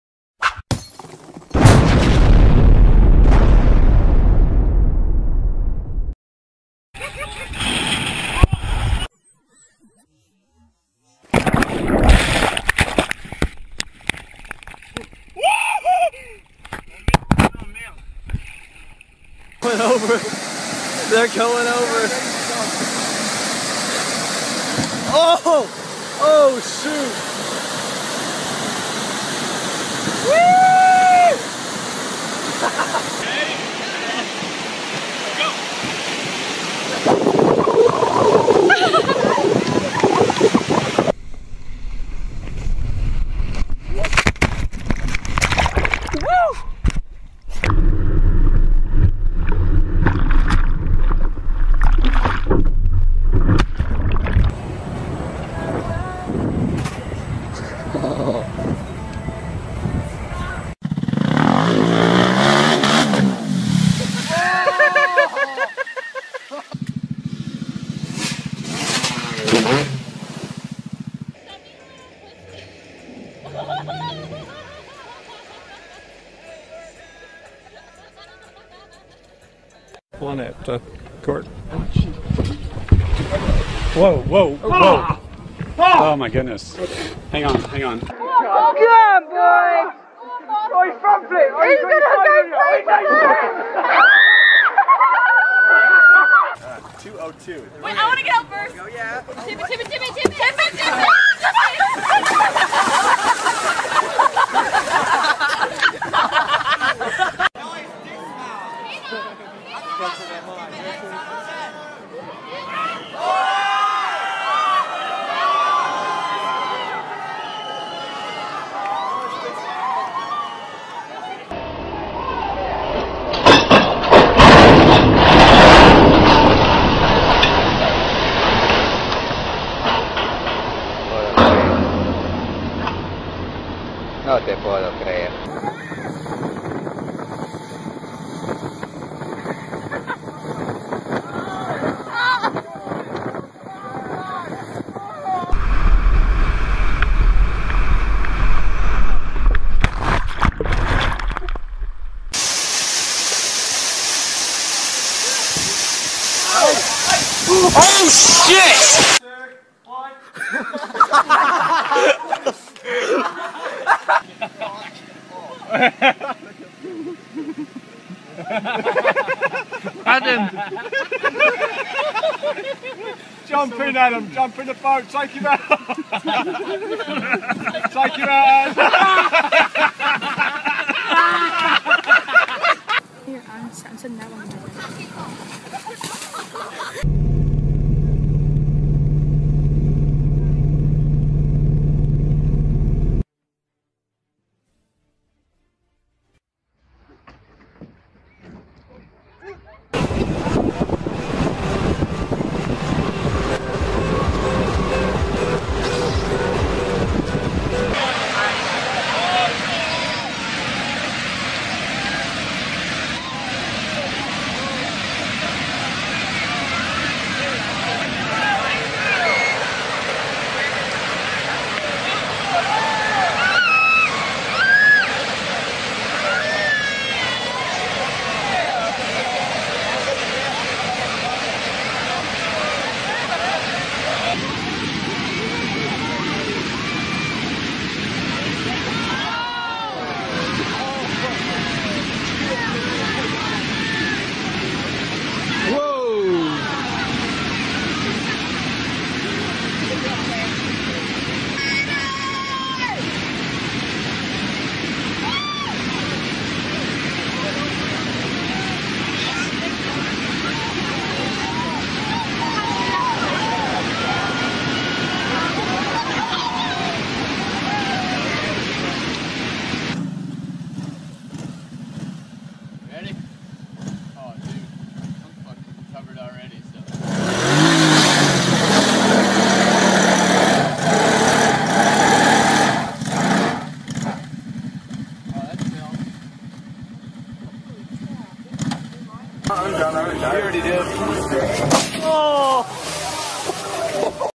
Water Fails | Water Disaster Compilation
Water fails. Water disater compilation. Just like on dry land, things can go terribly wrong in the water too, and the results are often hilarious.